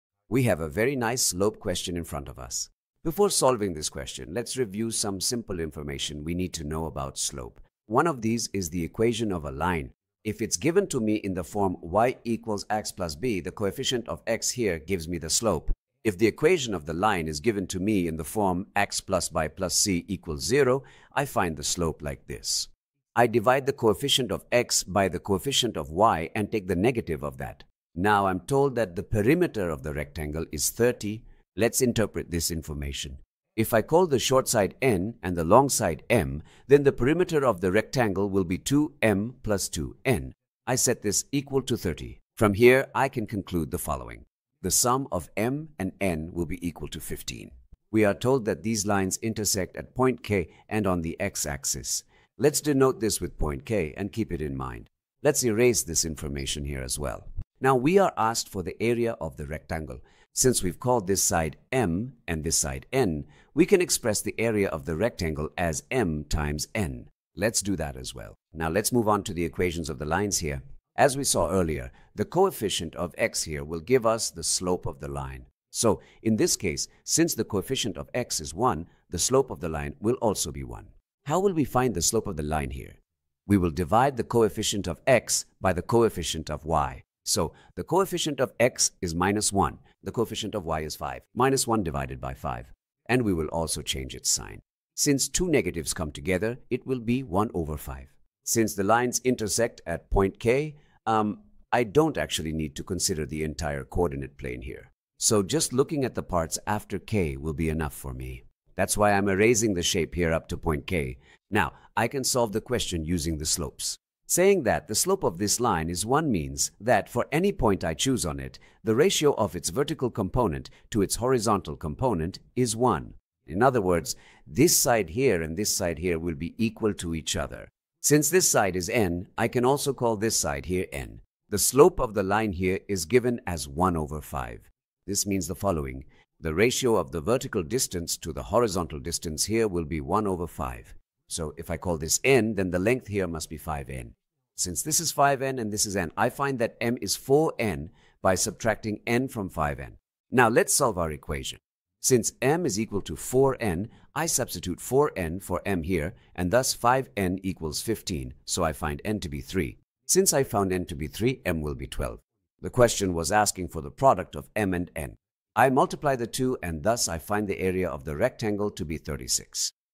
Only Those Who Can Read Slopes Will Solve This Rectangle Area! 📐🔥
Two lines intersect at a point on the x-axis.
One passes through the rectangle’s top-right corner, the other through the top-left corner.
One line hides its slope in the form
y=ax+b, while the other hides it in
ax+by+c=0.
You are given the perimeter of the rectangle, and using these two lines, we uncover its area.
A perfect problem to understand slopes, line forms, and geometry together.
I solved it step-by-step in under 3 minutes — don’t miss it! ✨📏